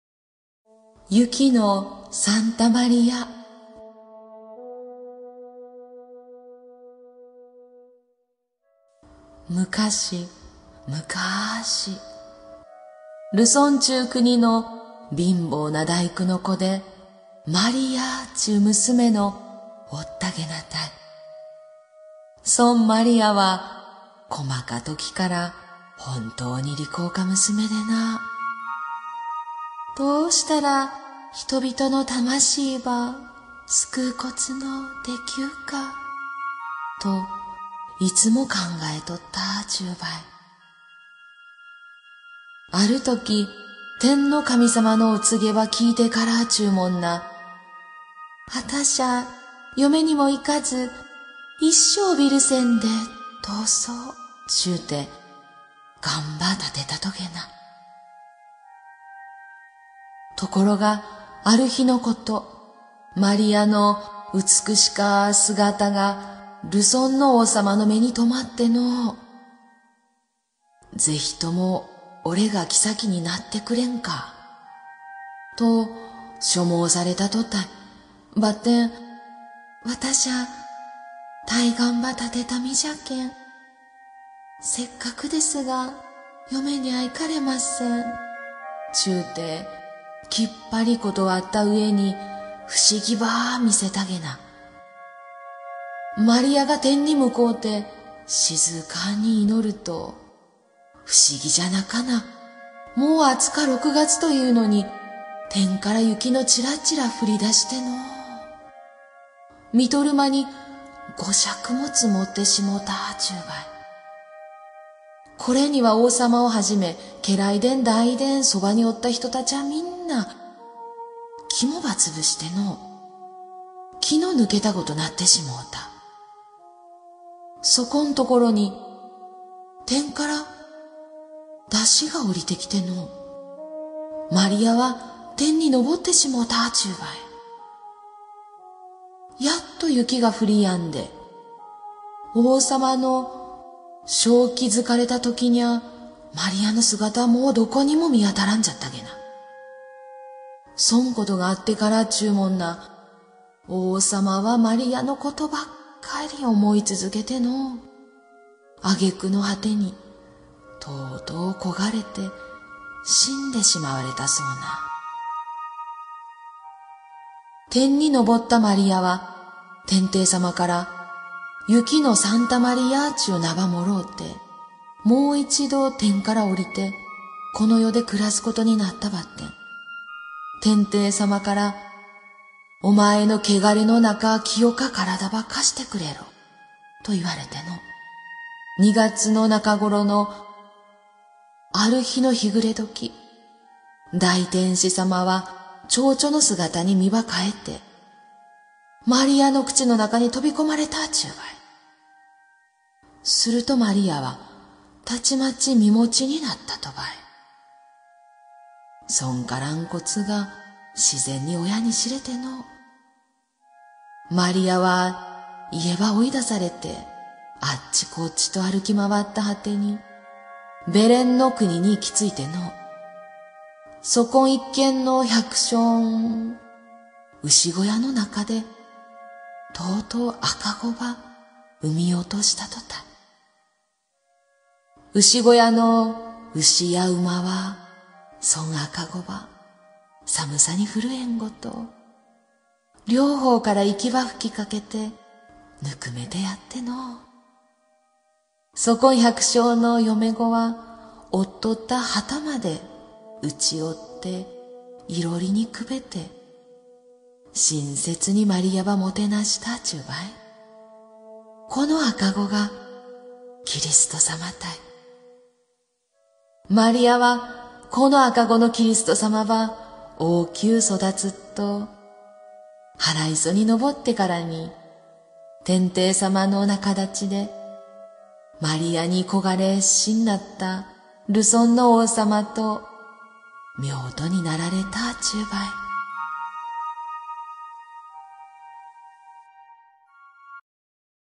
【長崎の民話】第8話1雪のサンタマリア
長崎弁で語られるキリスト生誕譚です。いわゆる聖書物語等に描かれるものとは、趣きが違っておりますが、こうやって300年近く伝承されていたのでしょうね。